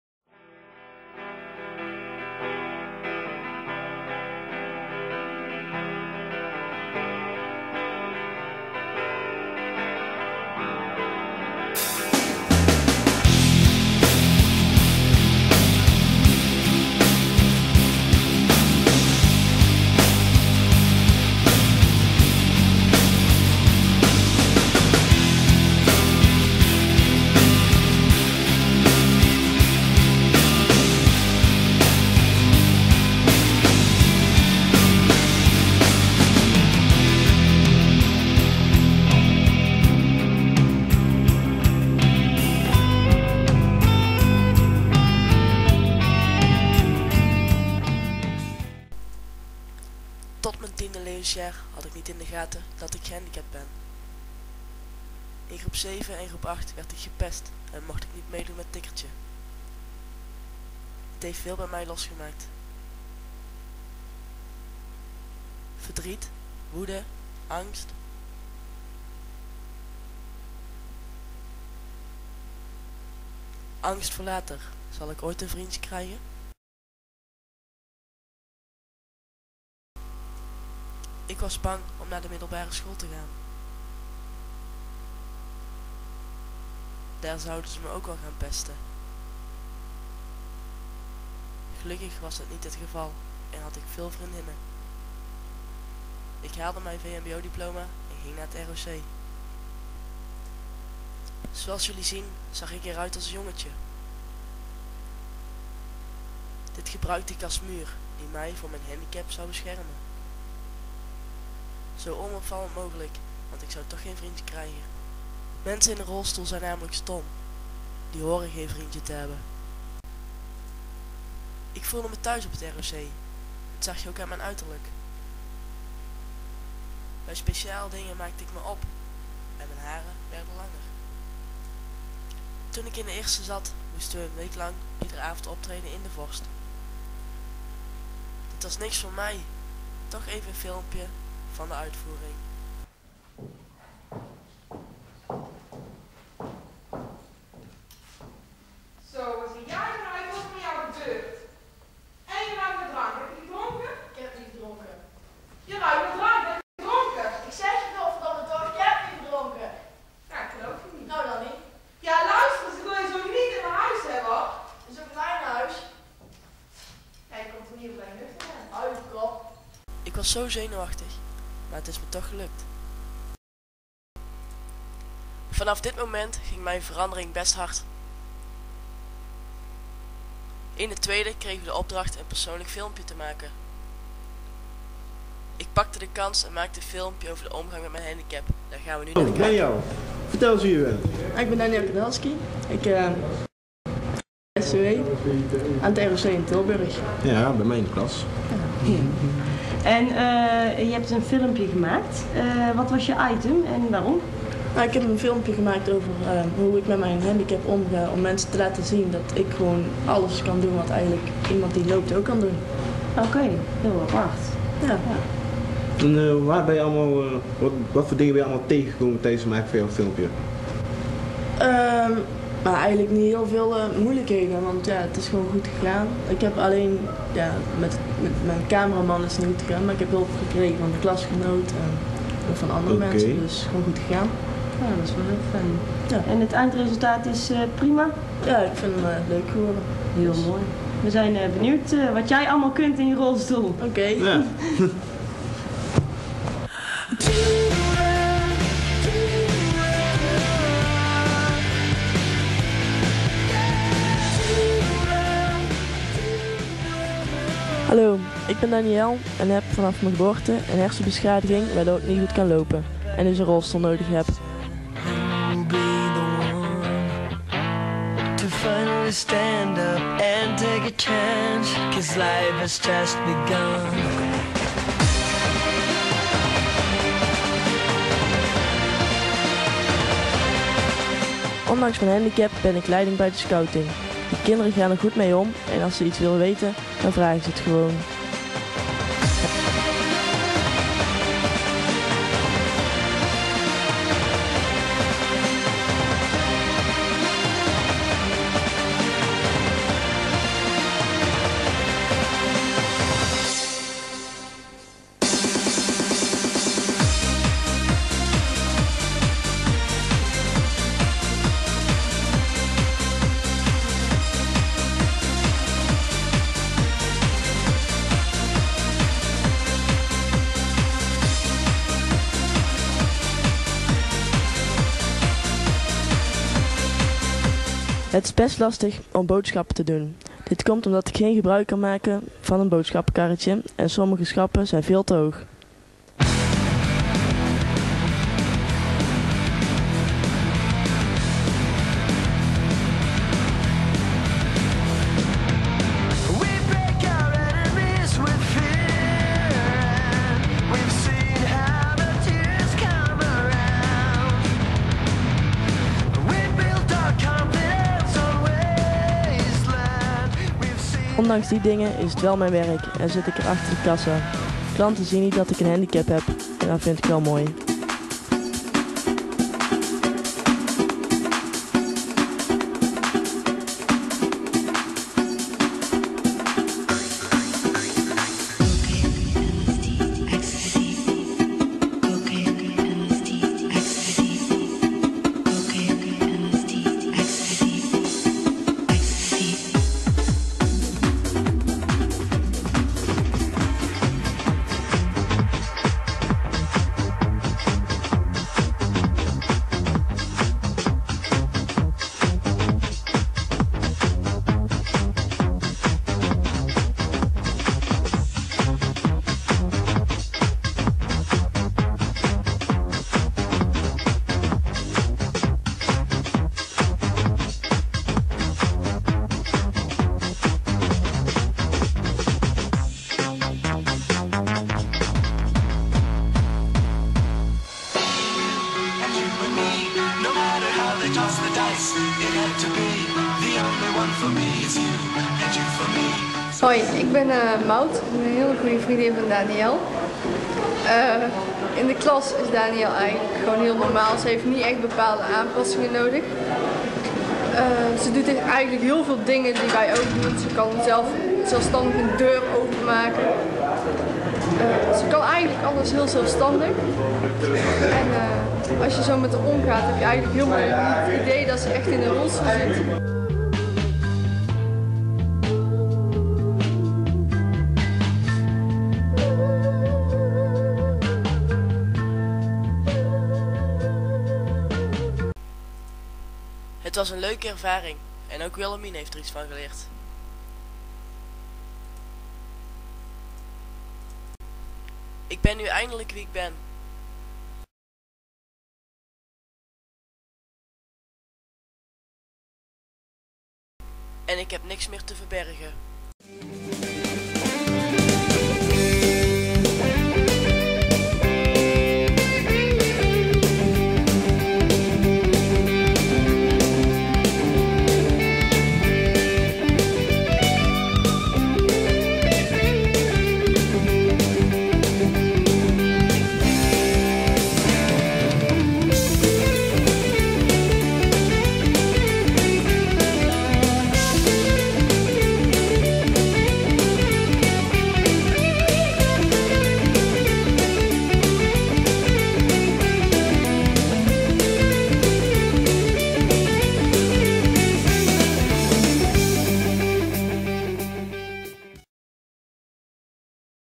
acceptation of handicap
this is a movie (in dutch) for school. With this I can let my classmates see how it is to have a disability...and what I was thinking at the age of 12 till 18